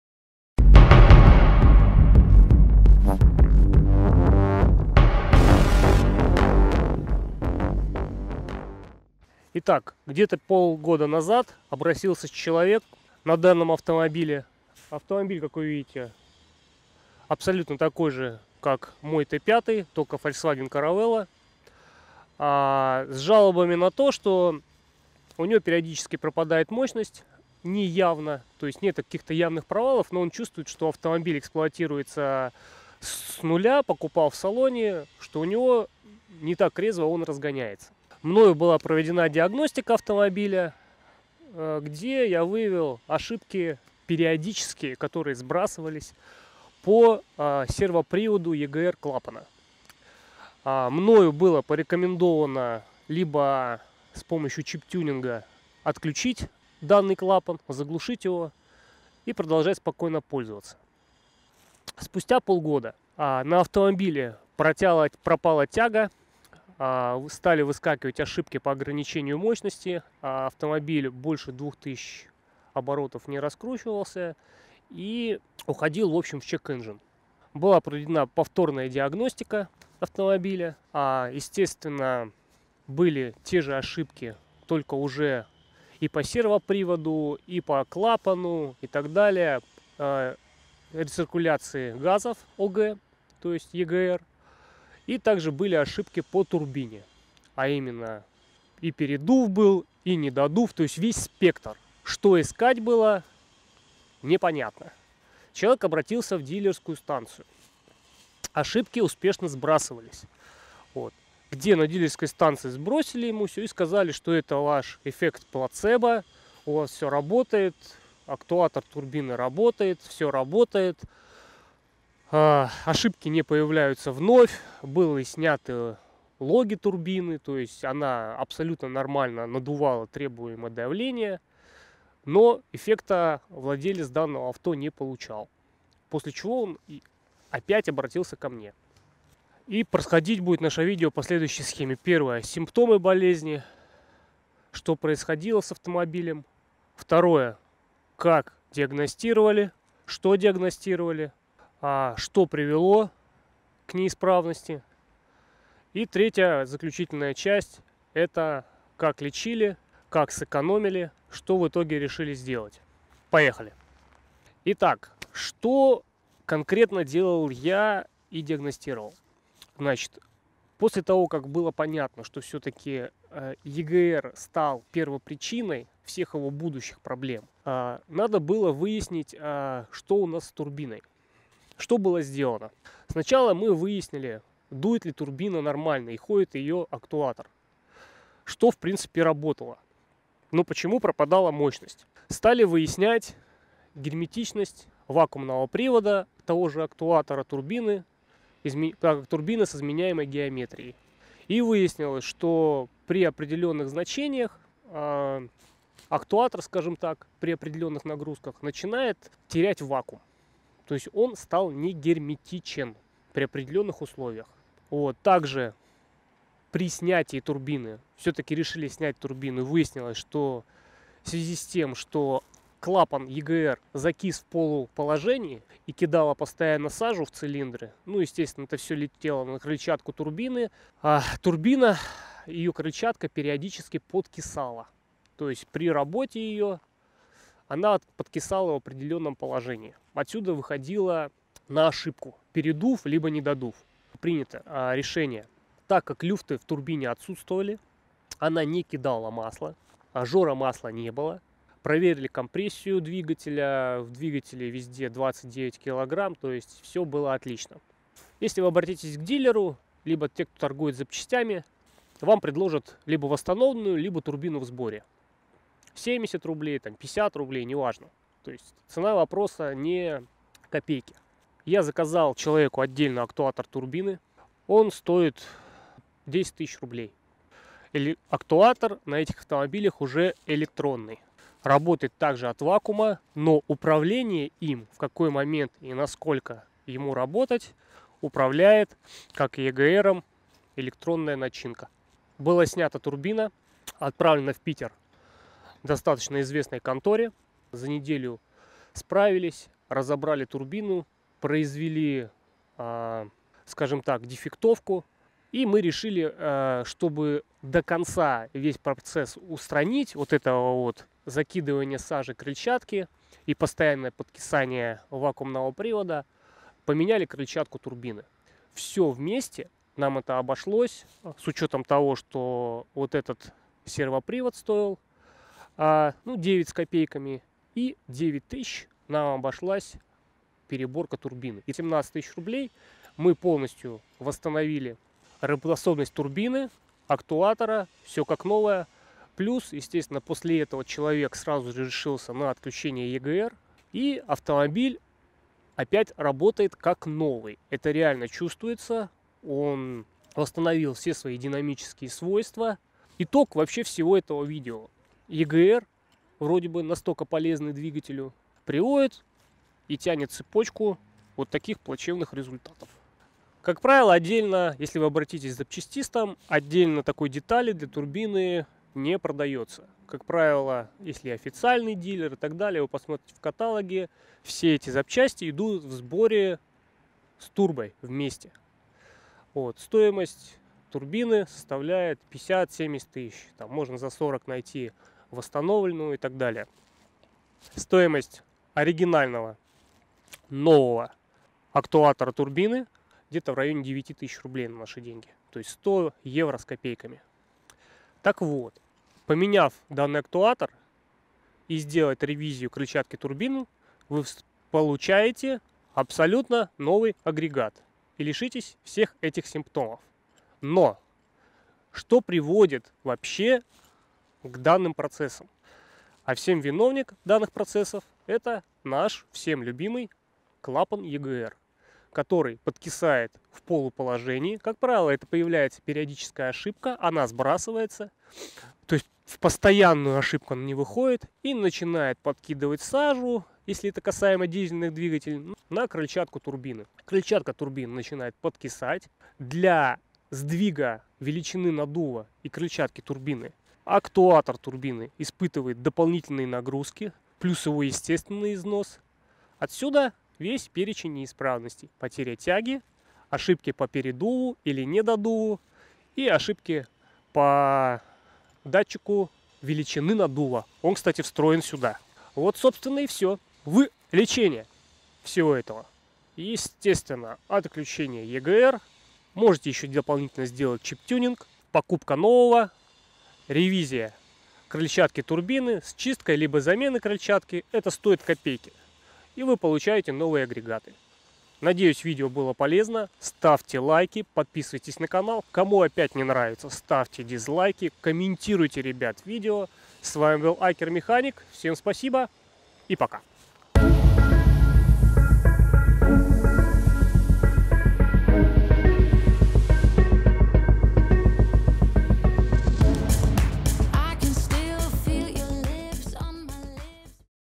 ЕГР(EGR) клапан УБИВАЕТ мотор ВАШЕГО авто!
Как Вы Убиваете Мотор  не отключая Клапан ЕГР (EGR)  автомобиля  который выводит из строя турбину. Как продлить жизнь мотору и увеличить ресурс ДВС
Все видео материалы являются собственностью автора. Ссылка на канал обязательна